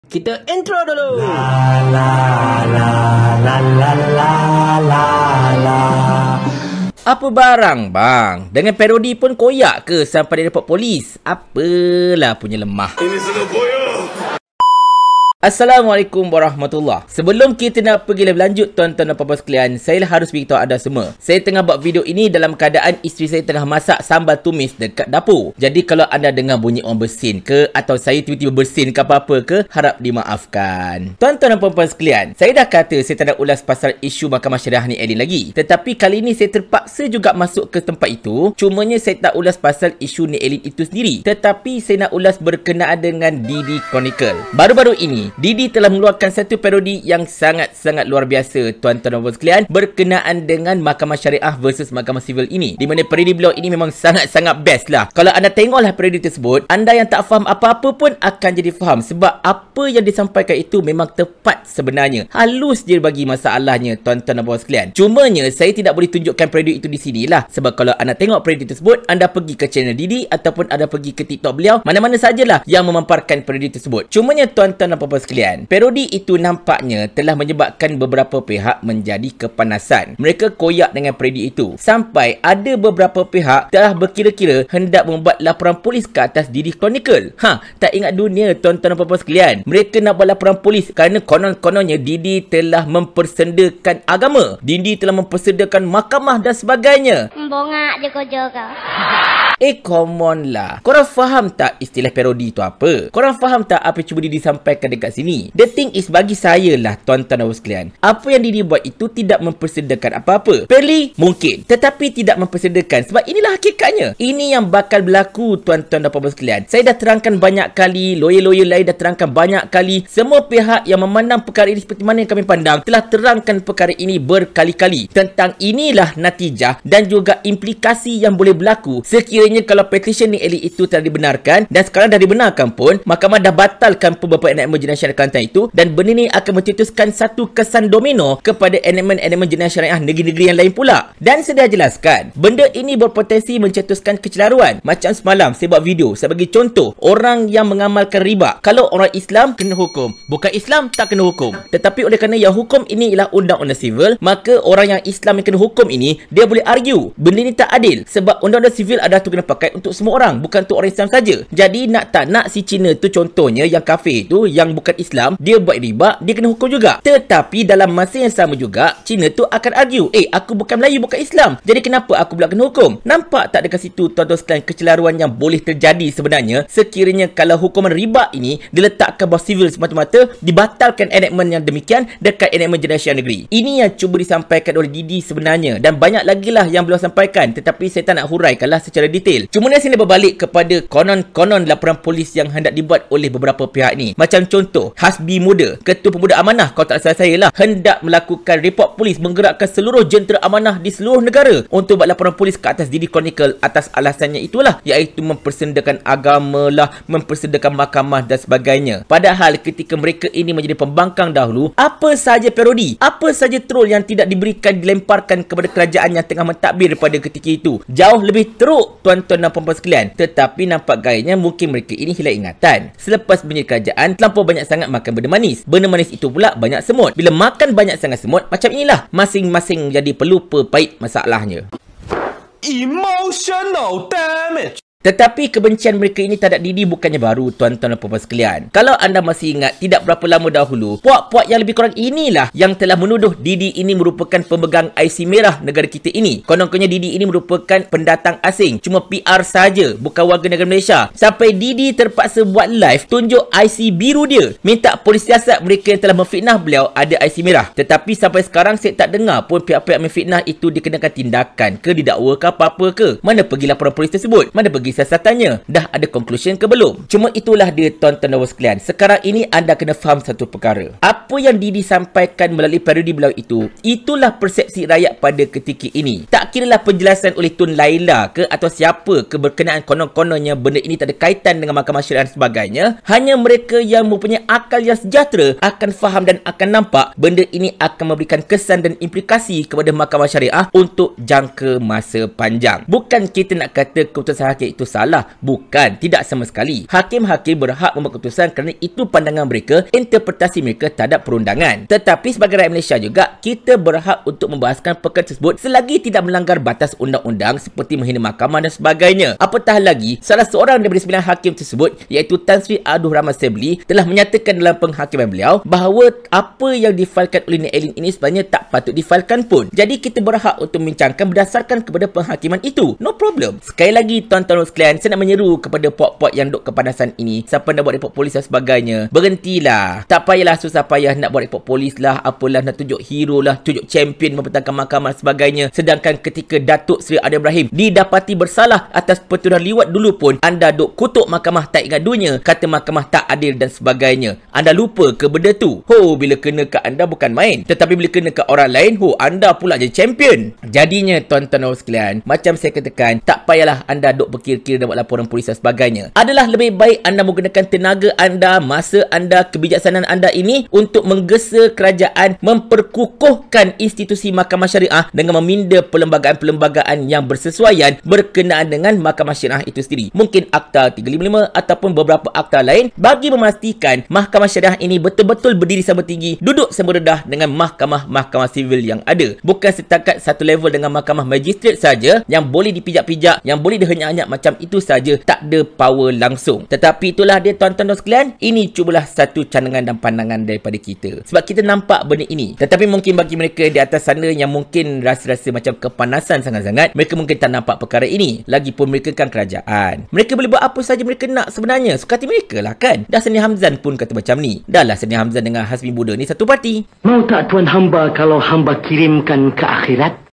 KOYAK sungguh mereka dengan parodi SAMPAI NAK BUAT REPORT POLIS!
kalau dengan parodi pun koyak, tak payah join politik la dik. BIKIN MALU